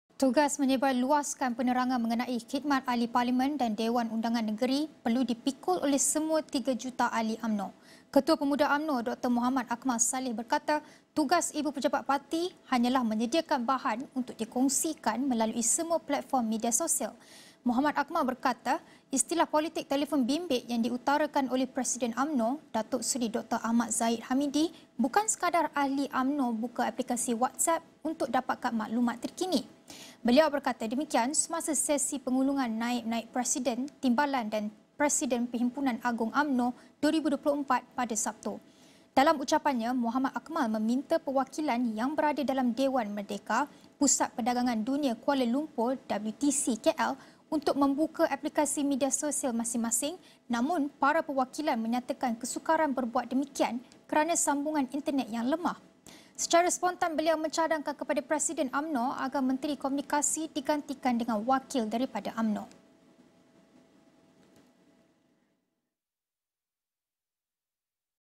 PAU 2024: Tugas penerangan perlu dipikul 3 juta ahli UMNO
Ketua Pemuda UMNO, Dr. Muhammad Akmal Saleh berkata tugas untuk memberikan penerangan kepada orang ramai perlu dipikul oleh semua 3 juta ahli UMNO.